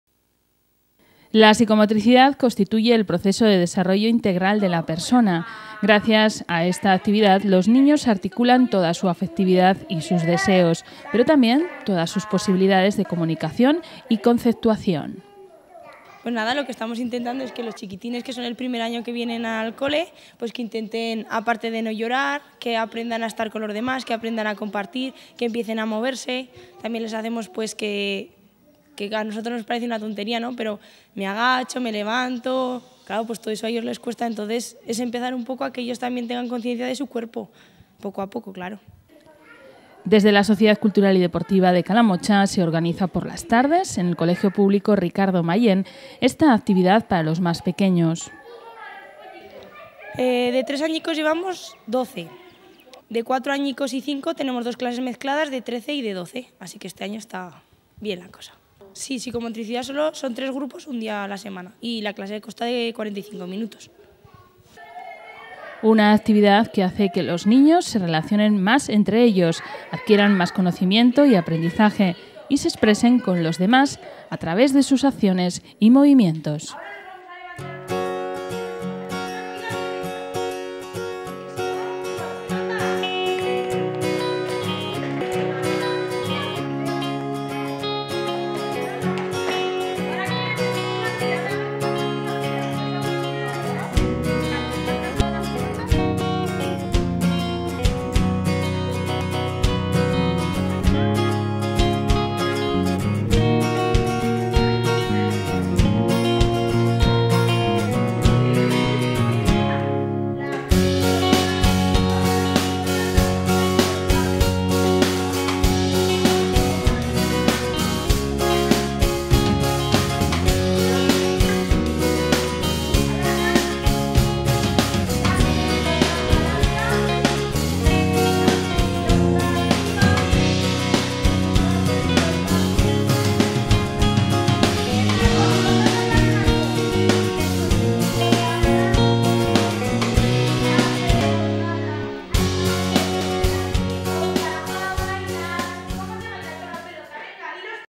Actividad física para los más pequeños.
La SCD también oferta actividad para los más pequeños de la casa.